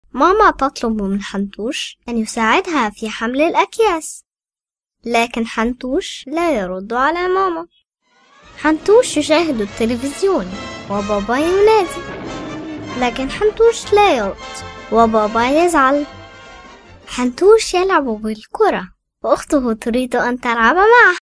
2423-Hira Arabic female voice talent - Narrative voice over
Hira is a 36 old Arabic female voice over talent. She has been in VO business for 11 years. Her primary jobs are interactive voice response systems, narrations and public announcements. Her natural voice type is adult. Her voice is most suitable for corporate and overdub, but can easily do numerous voice styles, such as amusing, commercials and sensitive as well as hypnotic, lively and tough moods.